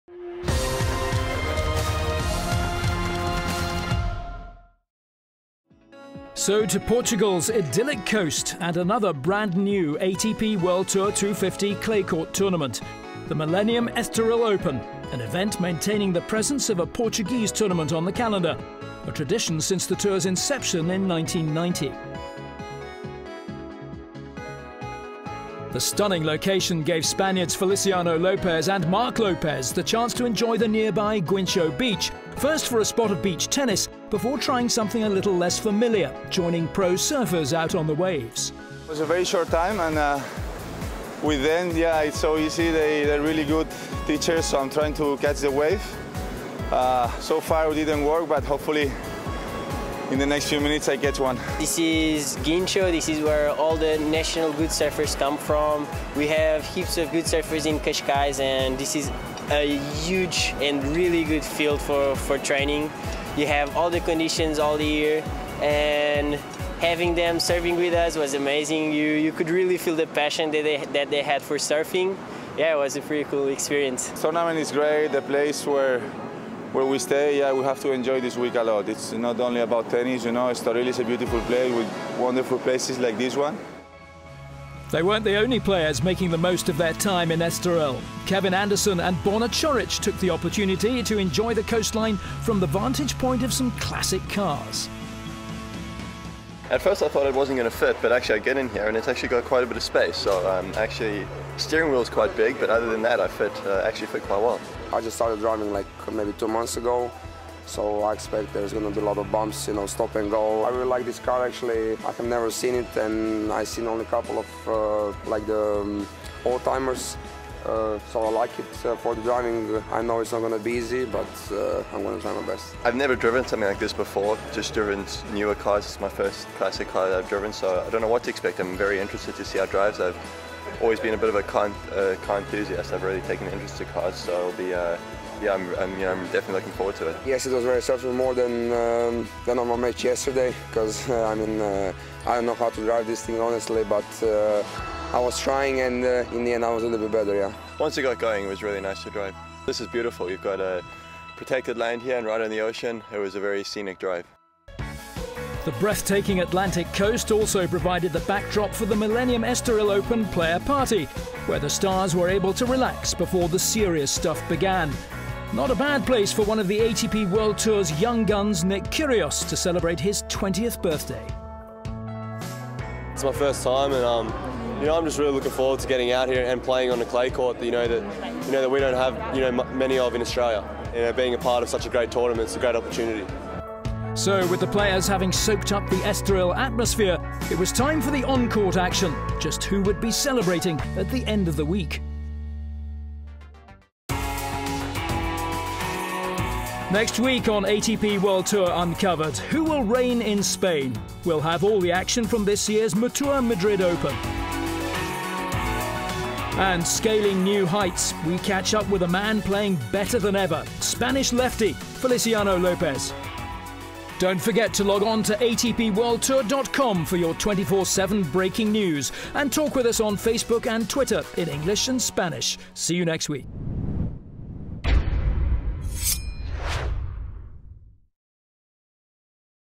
ATP World Tour Uncovered Estoril
Go behind the scenes at the inaugural Estoril tournament, where Marc Lopez and Feliciano Lopez tried surfing.